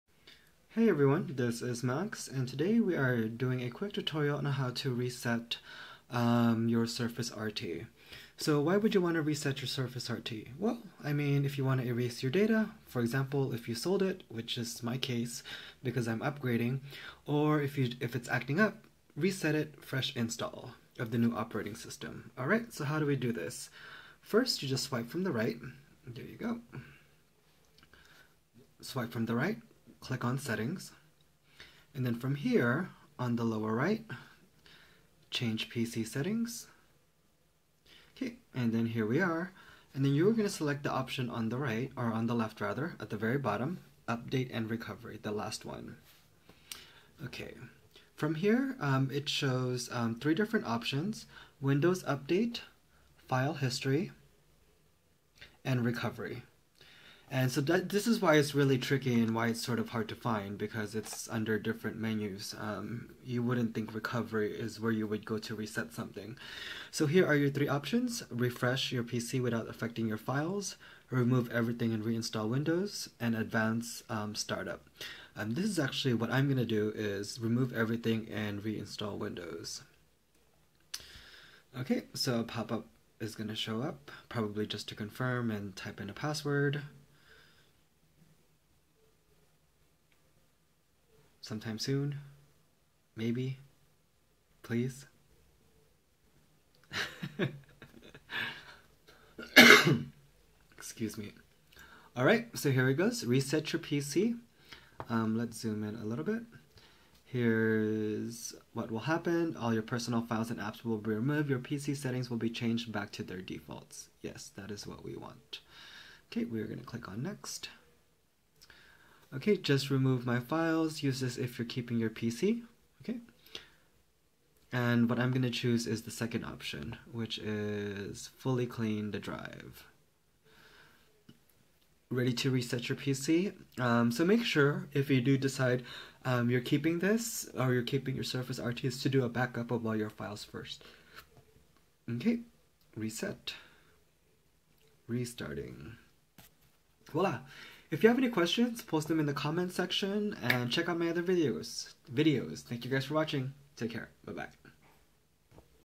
RESET - Microsoft Surface RT, Pro 1, 2 or 3 (ANY Windows 8.1 Tablet)
Aloha everyone,

Today we record how to WIPE your Surface RT or your Surface Pro of all data.  You should perform this step when selling or giving your Surface RT/Pro to another person!  This will also work on any Windows 8.1 Tablet (Toshiba Encore, Dell Venue, and more!)